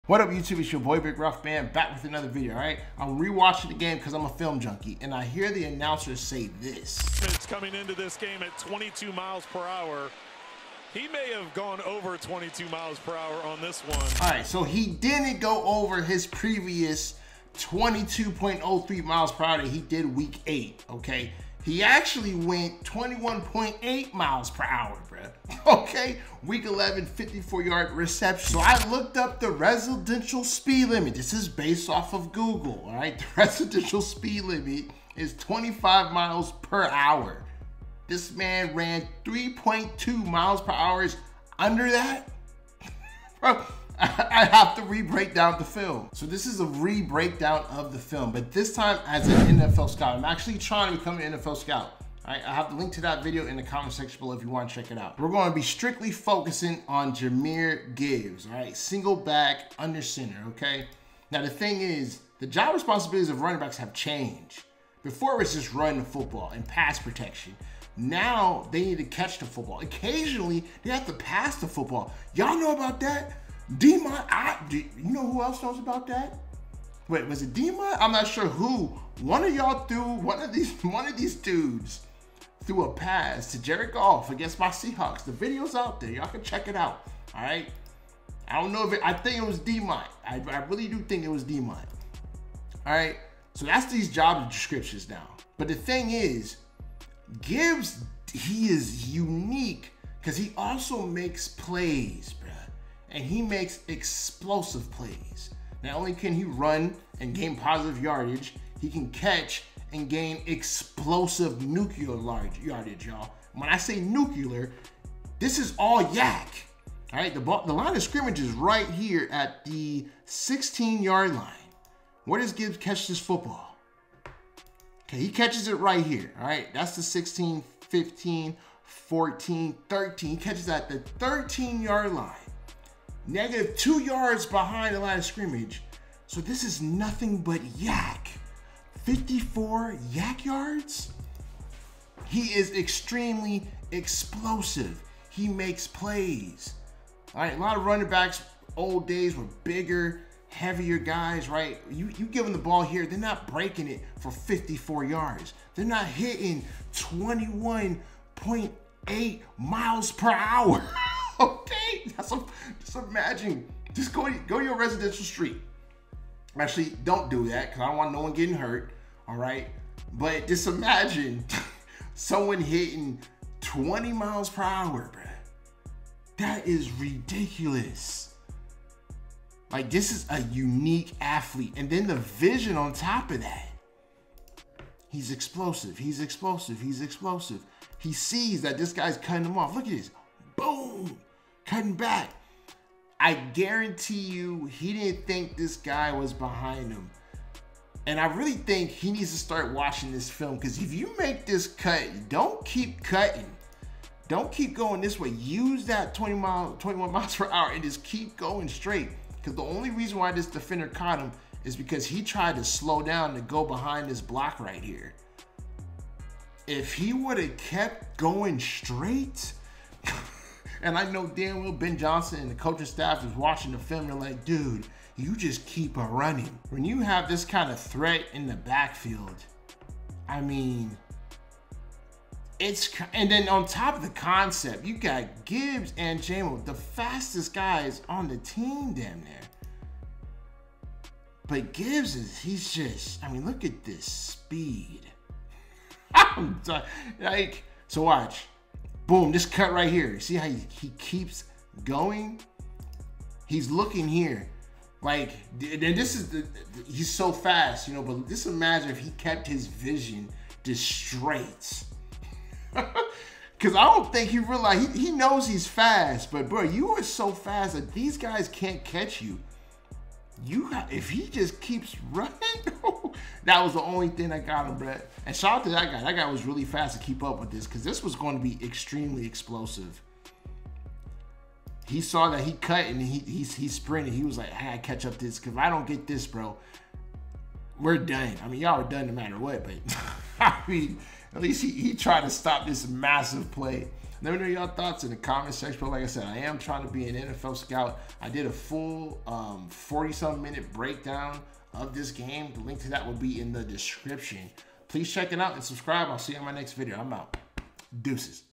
Watching Jahmyr Gibbs RUN 3.2 MPH under the OFFICIAL residential Speed Limit | Jaguars vs Lions
Link to Full reaction breakdown of game: Seahawk fan reacts to Jacksonville Jaguars vs. Detroit Lions | NFL 2024 Week 11 | BigR